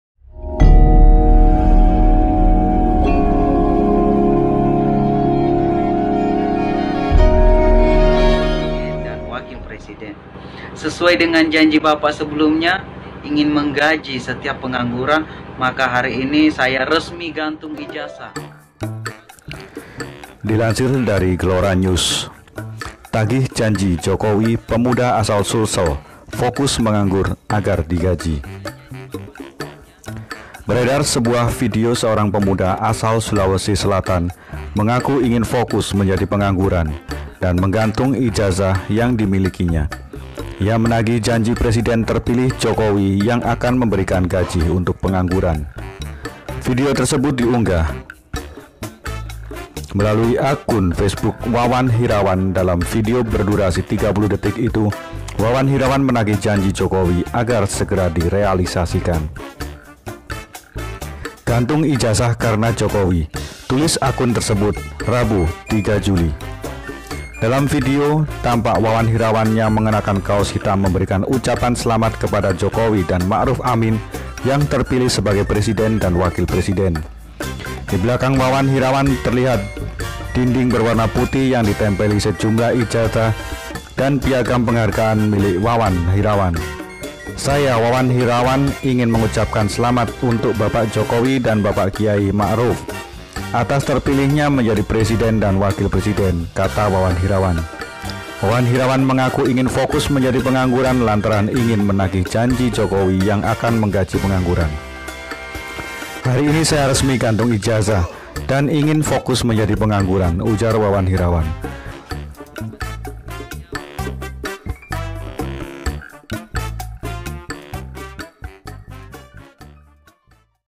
VIRAL!! PENGANGGURAN DIGAJI!! WAWAN HIRAWAN GANTUNG IJAZAH TAGIH JOKOWI
Beredar sebuah video seorang pemuda asal Sulawesi Selatan mengaku ingin fokus menjadi pengangguran dan menggantung ijazah yang dimilikinya. Ia menagih janji Presiden Terpilih Jokowi yang akan memberikan gaji untuk pengangguran.

Video tersebut diunggah melalui akun Facebook Wawan Hirawan. Dalam video berdurasi 30 detik itu, Wawan Hirawan menagih janji Jokowi agar segera direalisasikan.

"Gantung ijazah karena Jokowi," tulis akun tersebut, Rabu (3/7/2019).

Dalam video, tampak Wawan Hirawan yang mengenakan kaos hitam memberikan ucapan selamat kepada Jokowi dan Maruf Amin yang terpilih sebagai presiden dan wakil presiden.

Di belakang Wawan Hirawan terlihat dinding berwarna putih yang ditempel sejumlah ijazah dan piagam penghargaan milik Wawan Hirawan.

"Nama saya Wawan Hirawan ingin mengucapkan selamat untuk Bapak Jokowi dan Bapak Kyai Haji Maruf Amin atas terpilihnya menjadi presiden dan wakil presiden," kata Wawan Hirawan.

Wawan Hirawan mengaku ingin fokus menjadi pengangguran lantaran ingin menagih janji Jokowi yang akan menggaji pengangguran.

"Hari ini saya resmi gantung ijazah dan ingin fokus menjadi pengangguran," ujar Wawan Hirawan.